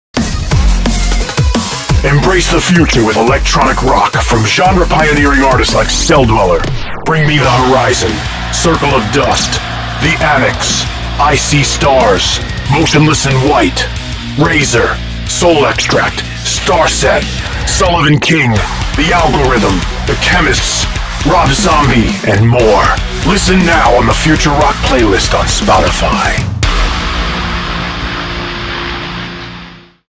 Future Rock Spotify Playlist
Embrace the future with electronic-rock from genre pioneering artists like Celldweller, BRING ME THE HORIZON, Circle of Dust, The Anix, I See Stars, Motionless In White, Raizer, Soul Extract, STARSET, Sullivan King, The Algorithm, The Qemists, Rob Zombie and more! What other artists would you like to see us add to our playlist?

Follow FiXT!